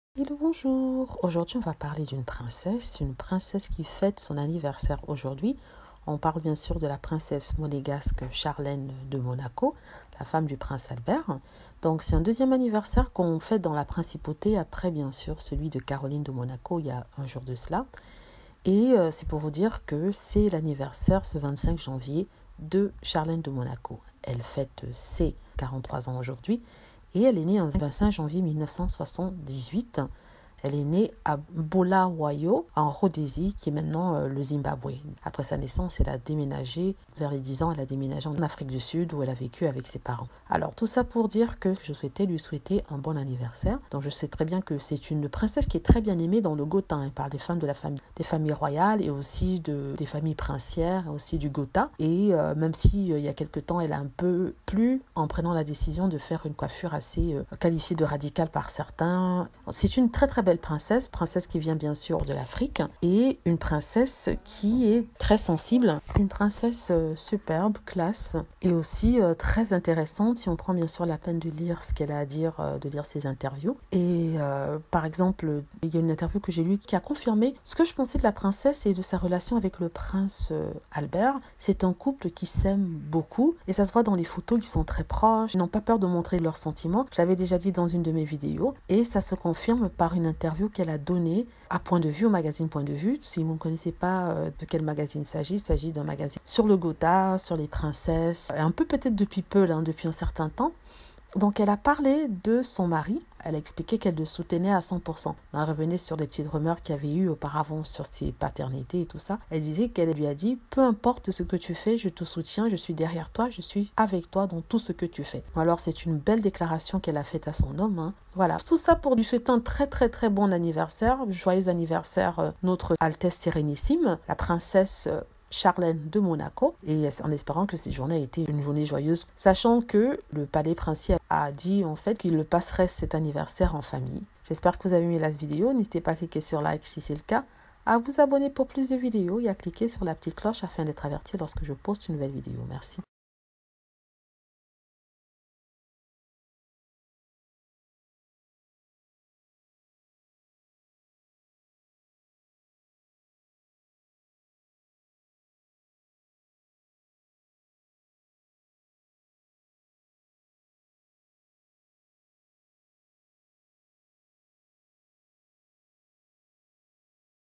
La Princesse Charlene fête ses 43 ans. Elle les fête en petit comité
aujourdhui 25 Janvier est une date d'anniversaire speciale. en tout cas pour une princesse monegasque. il s'agit de la Prince Charlene la femme du Prince Albert grimaldi qui fete ses 43 ans. Elle est née charlene winstock en Rhodesie.